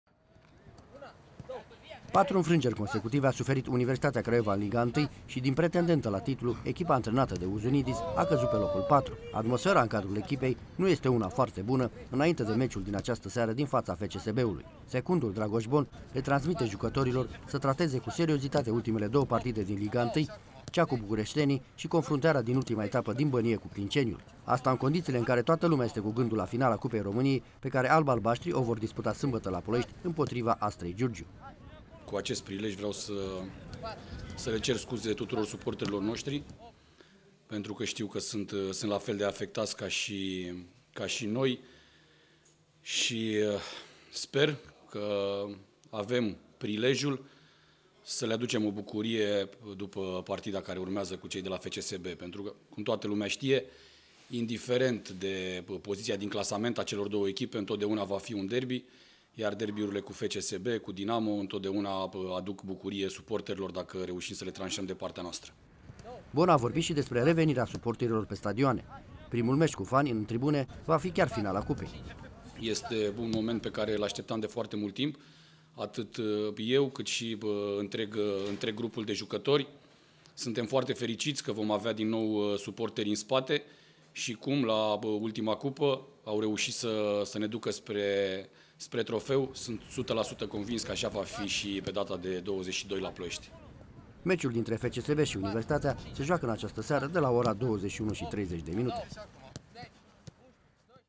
DERBY AL ORGOLIILOR
Universitatea joacă în penultima etapă în deplasare la rivala FCSB. Cum CFR a ieşit campioană matematic după victoria de la FC Botoşani, meciul dintre cele două formaţii va fi unul al orgoliilor. Oltenii trebuie să câştige dacă vor să termine acest sezon pe locul trei.

Patru înfrângeri consecutive a suferit Universitatea Craiova în Liga I şi, din pretendentă la titlu, echipa antrenată de Ouzounidis a căzut pe locul patru în Liga I. Atmosfera în cadrul echipei nu este una foarte bună înainte de meciul din această seară din faţa FCSB-ului. Secundul Dragoş Bon le transmite jucătorilor să trateze cu seriozitate ultimele două partide din Liga I, cu bucureştenii şi confruntarea din ultima etapă din Bănie cu Clinceniul. Asta în condiţiile în care toată lumea este cu gândul la finala Cupei României pe care alb-albaştrii o vor disputa sâmbătă la Ploieşti împotriva Astrei Giurgiu.

Bon a vorbit şi despre revenirea suporterilor pe stadioane. Primul meci cu fani în tribune va fi chiar finala Cupei.

Meciul dintre FCSB şi Universitatea se joacă în această seară de la ora 21.30.